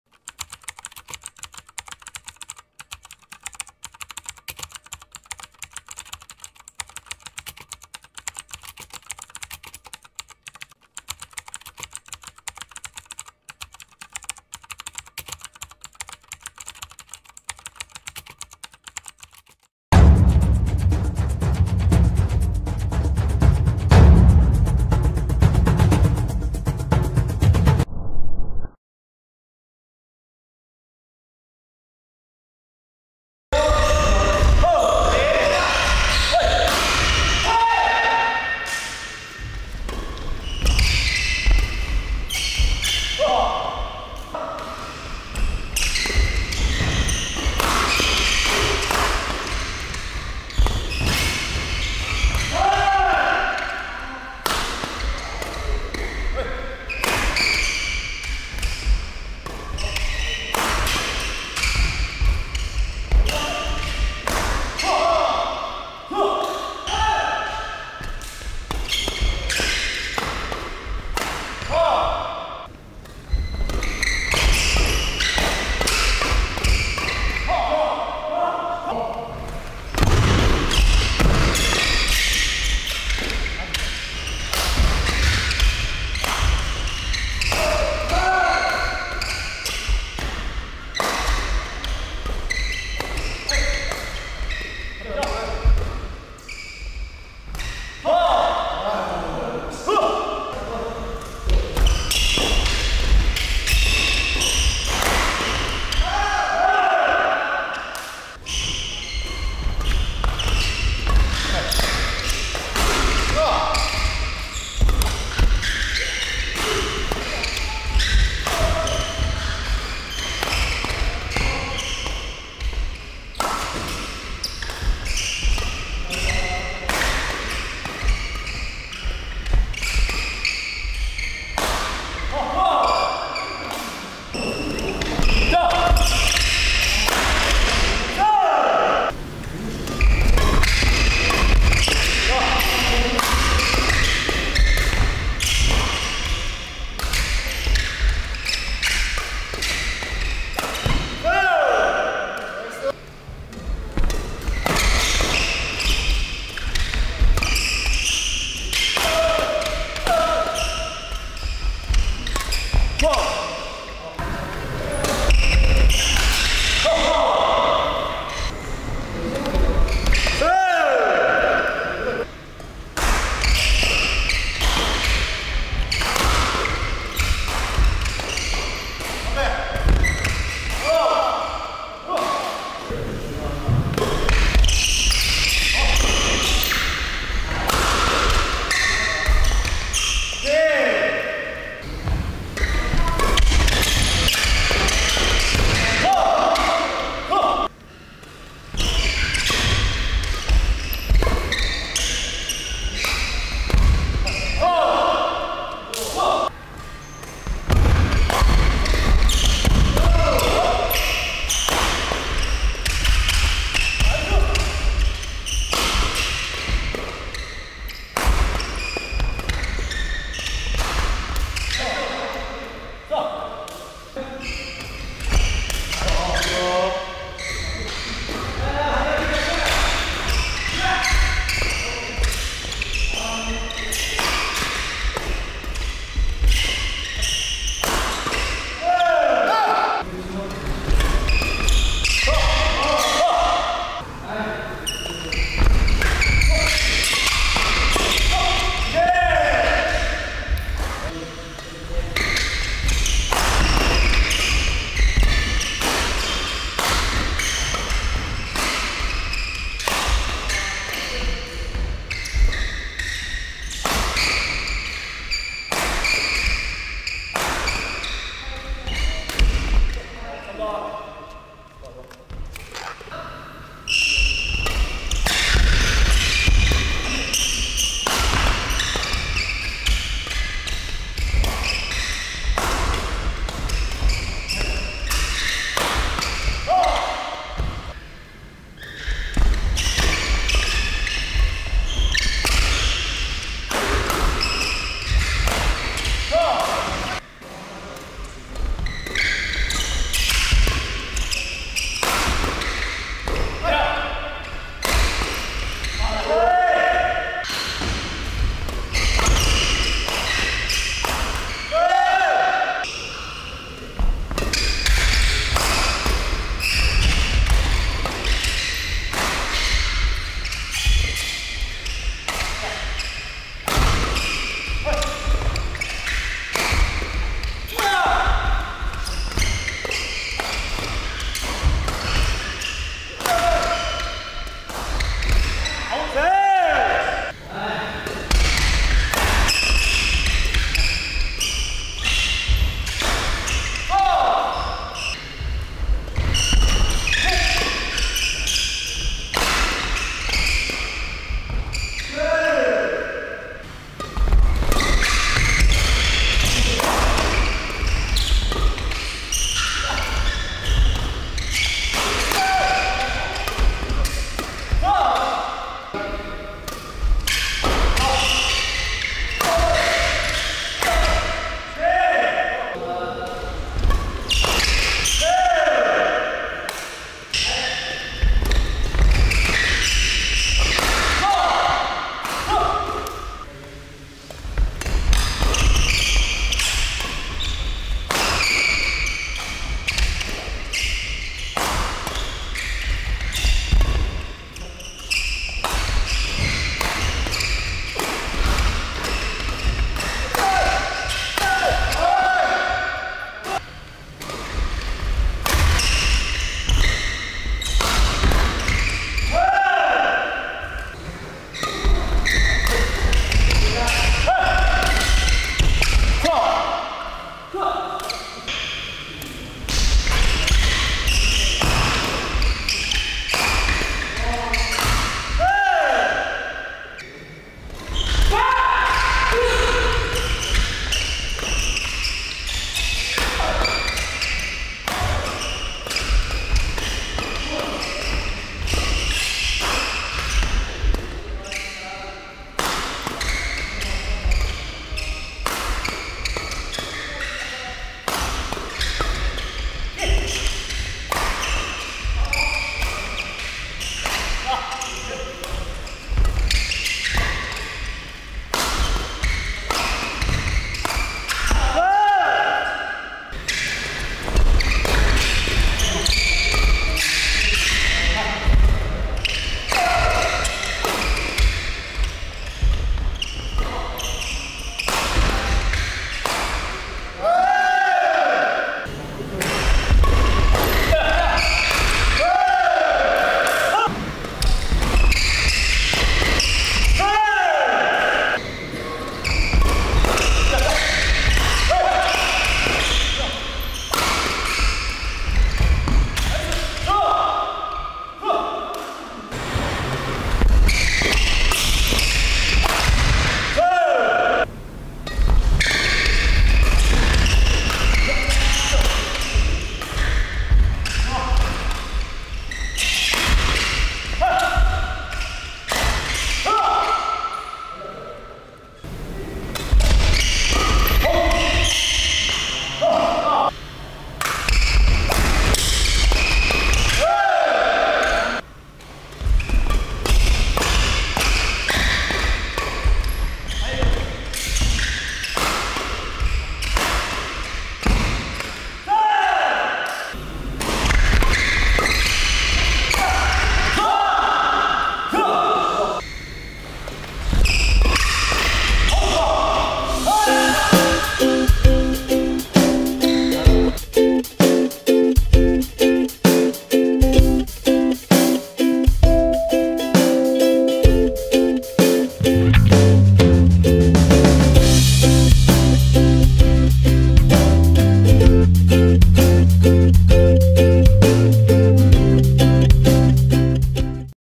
6조3경기 (팀 맥스)원해성.기성현 vs (CARRY)문영화.정춘수
제1회 정민턴배 전국 동호인 남복 최강전
예선 6조3경기! (팀 맥스)원해성.기성현 vs (CARRY)문영화.정춘수

1등 상금 50만원(2등 30만원) 에 도전하는 전국 동호인들의 치열한 경기!!
과연 누가 우승할 것인가?